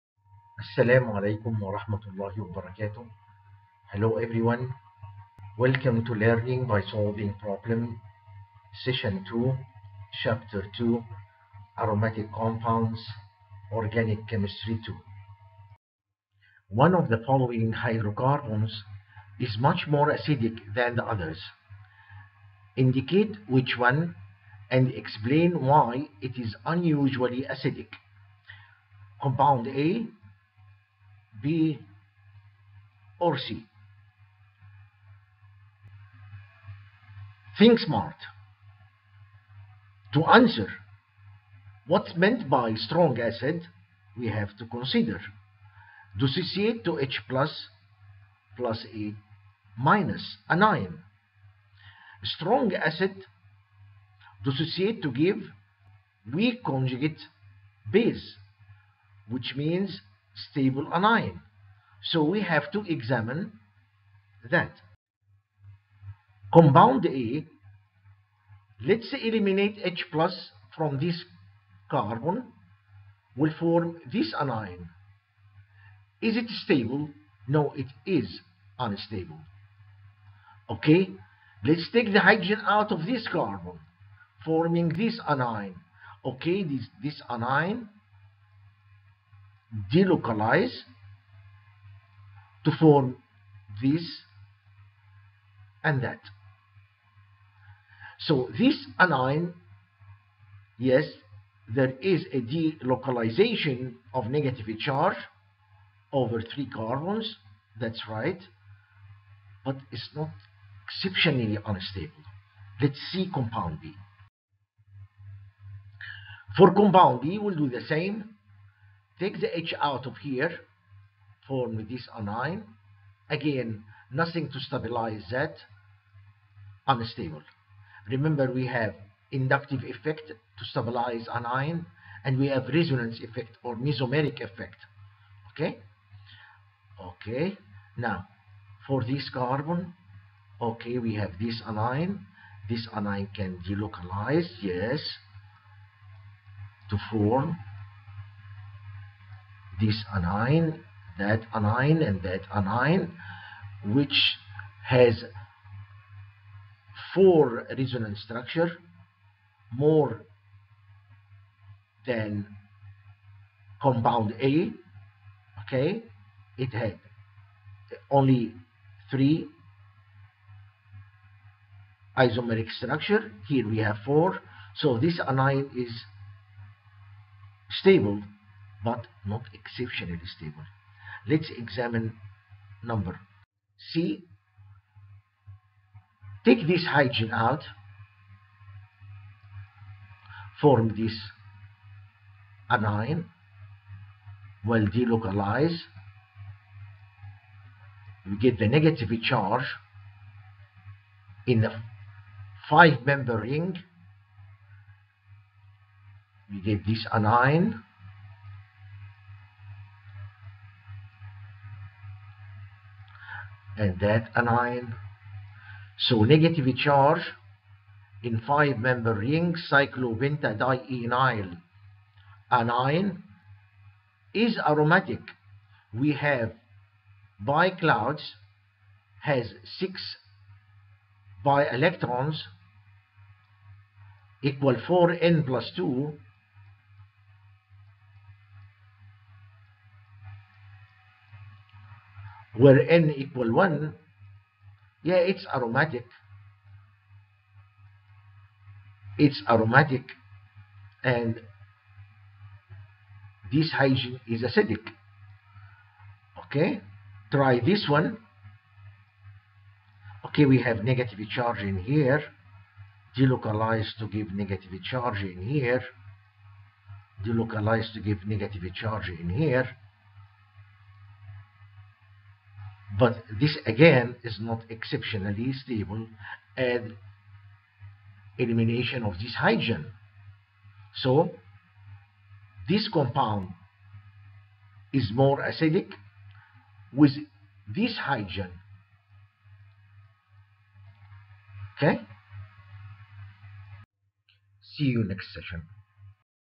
EN Learning by Solving Problems Chapter 2 2 Org Chem II
Amer's School of Organic Chemistry, Aromatic Ions, Acidity, Problem Solving Skill